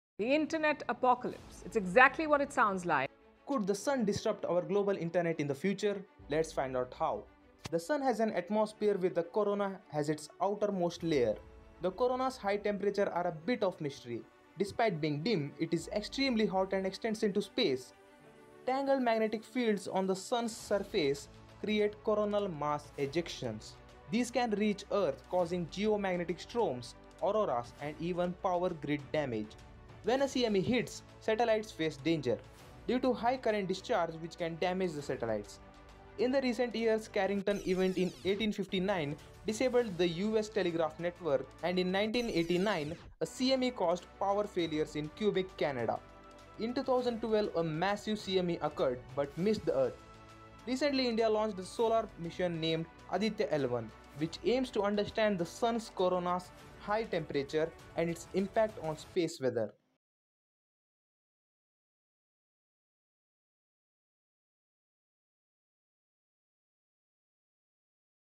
Can Geomagnetic Strom DESTROY Earth? know the SHOCKING Truth!
Long Back in 1900s A Geomagnetic Strom was hit to earth causing A EXPENSIVE damage to human Development and this is being Continued with a Regular Intervals and Many other KNOWLEDGELESS facts and truths are being discussed in this Video .

Thankyou 🫡